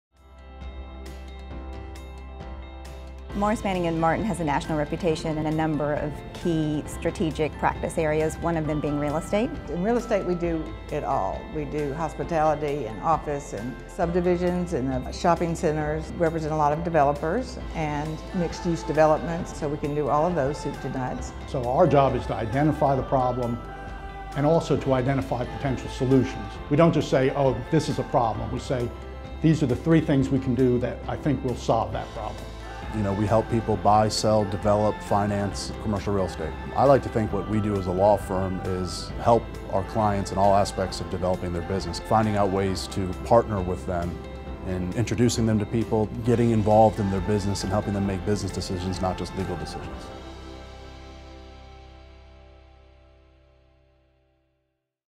MMM Commercial Real Estate Development & Finance Practice
The Commercial Real Estate Development & Finance Practice at MMM is one of the key original practices of the firm. For over four decades, our lawyers have represented developers and investors in every aspect of commercial real estate, including fund formation, zoning and other land use matters, acquisition, debt and equity financing, development and construction, environmental strategies, leasing, operation and disposition.

Our Real Estate team’s close cooperation with the firm's litigation, bankruptcy and insolvency, private equity, environmental, project finance, public sector, construction, tax and other practices enables us to deliver comprehensive services for virtually every kind of real estate need a client might have.